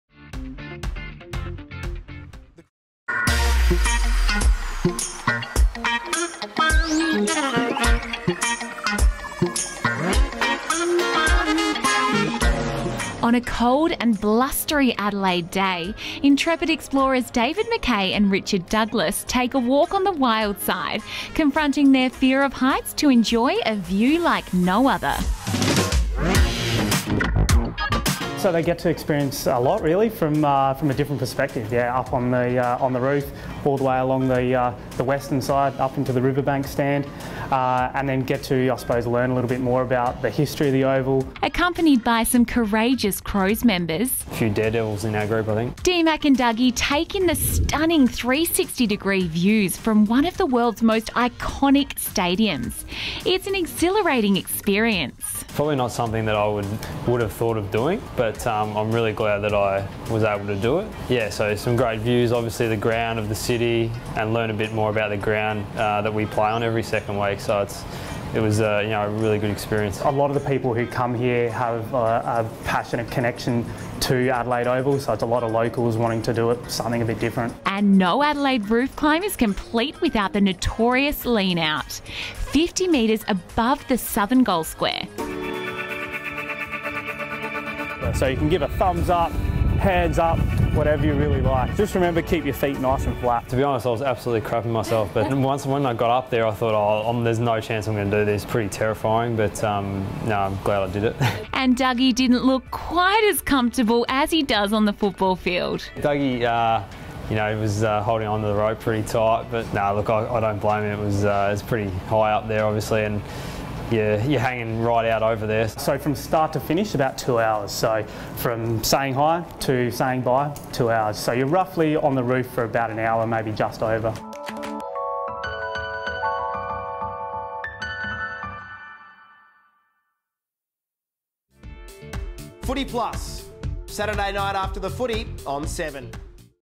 Dougy & D-Mac tackle the climb
Crows Midfielders Richard Douglas and David Mackay strap themselves in and take on the Adelaide Oval Roof Climb.